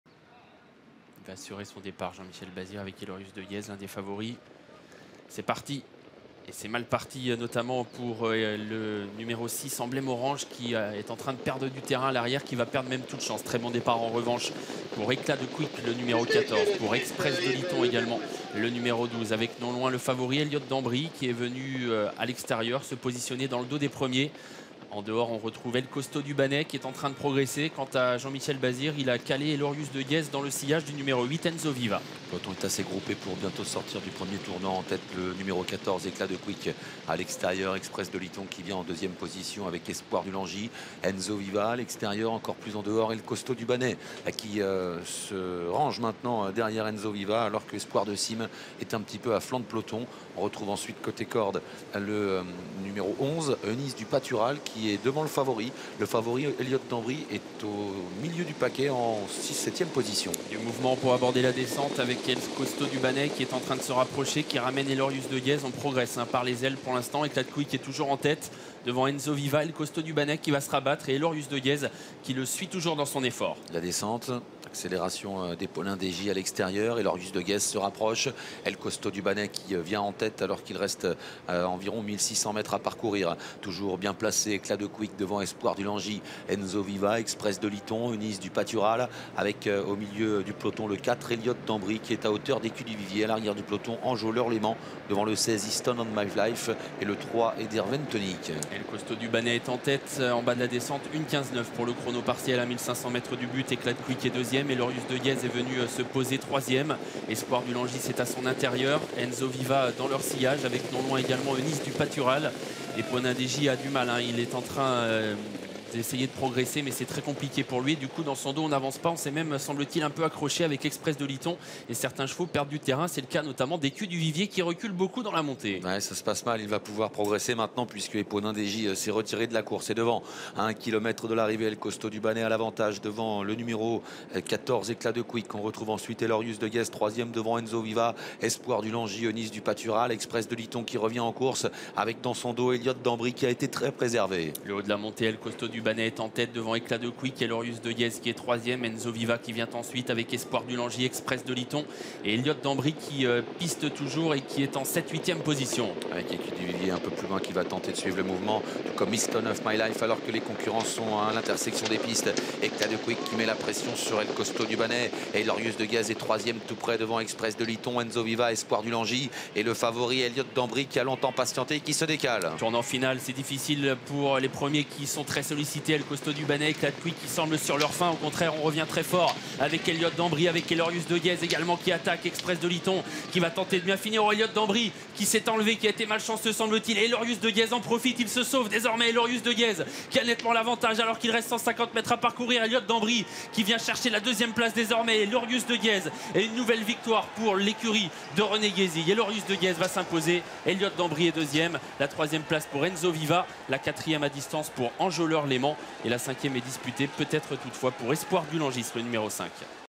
ELORIUS DE GUEZ | Arrivée du Quinté+ | 04/12/18
Arrivée du Quinté+ du 04/12/18 - Prix de Laon - Vincennes
Toutes les courses sur Equidia.fr
⬇ PLUS D'INFOS ⬇

Après avoir assuré son départ Jean-Michel Bazire rapproche dans la descente Elorius de Guez. A l'entrée de la ligne droite, il prend la mesure de ses rivaux et file vers un franc succès. Retardé par un court accrochage à la sortie du dernier tournant, Eliot d'Ambri refait du terrain sur le partenaire de JMB, sans avoir le temps de le menacer. Enzo Viva s'empare du second accessit en retrait, tandis qu'Enjoleur Léman finit vite pour ravir la quatrième place à Espoir du Langis, qui a toujours figuré en bon rang. Après enquête, Espoir du Langis est distancé pour s'être déporté de la corde à la sortie du dernier tournant, gênant plusieurs rivaux. Exil d'Erable complète donc la combinaison gagnante du quinté.